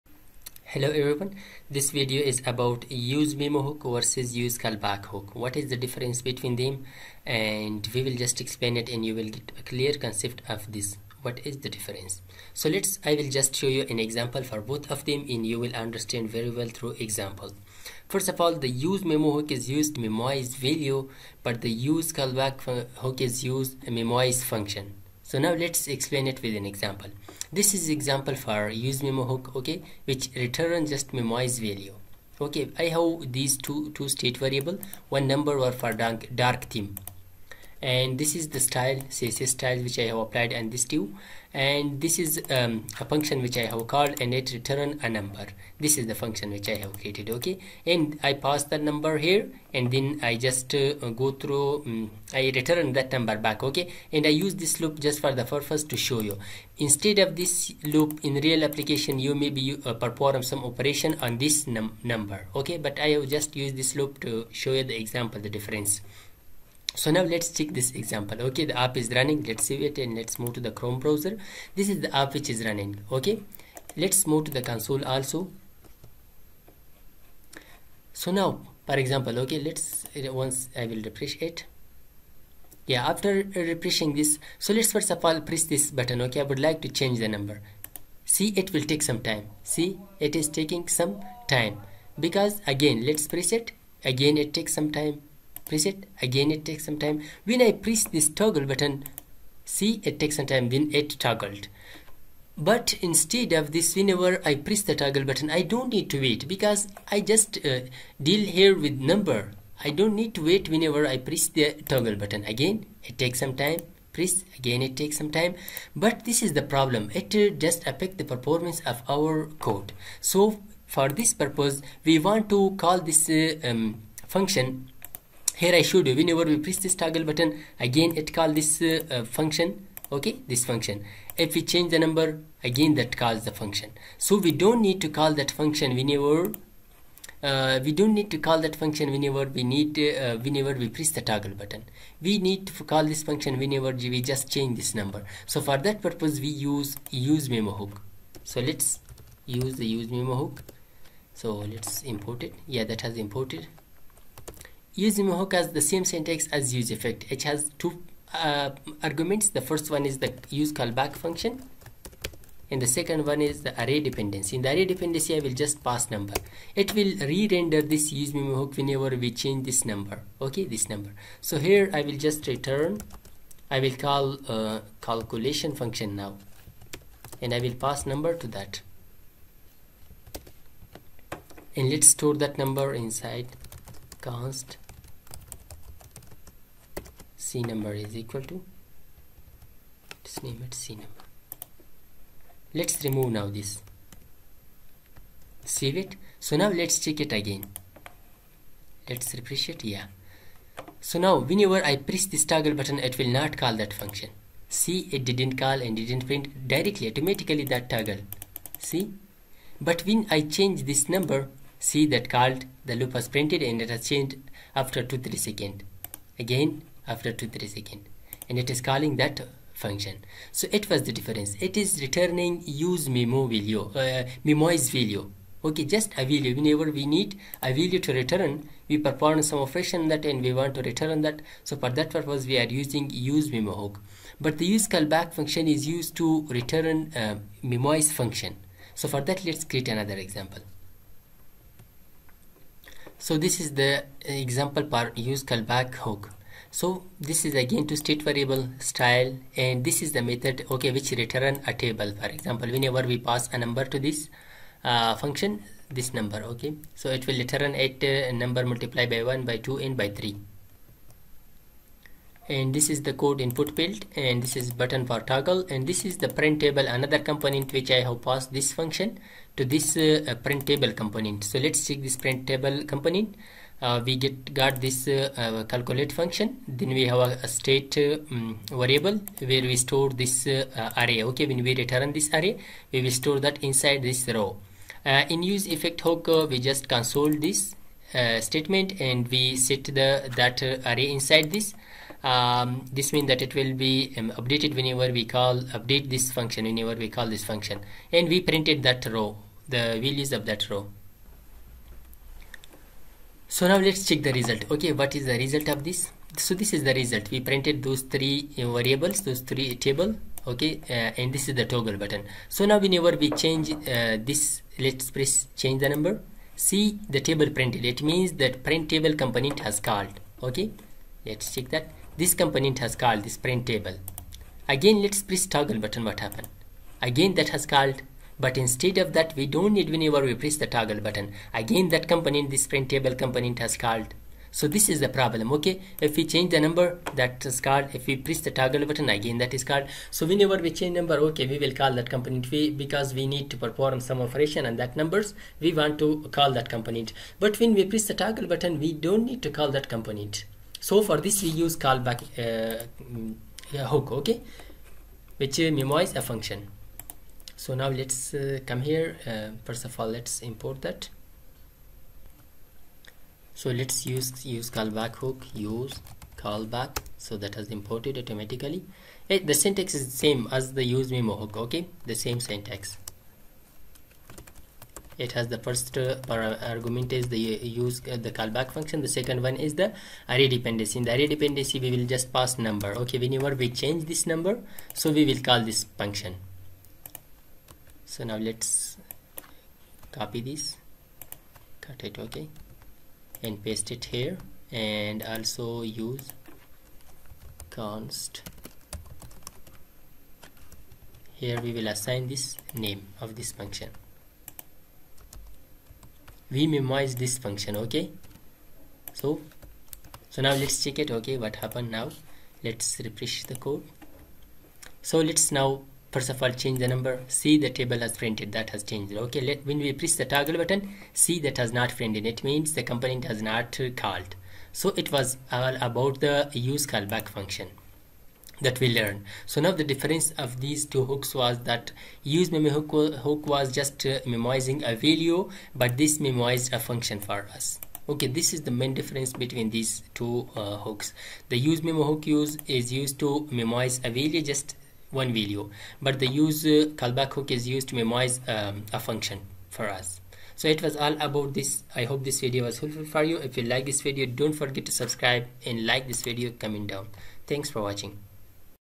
useMemo vs useCallback hook in react JS
This video is about useMemo vs useCallback hook in react js. The difference between theses two hooks is that useMemo memoize value buth useCallback hook memoize function.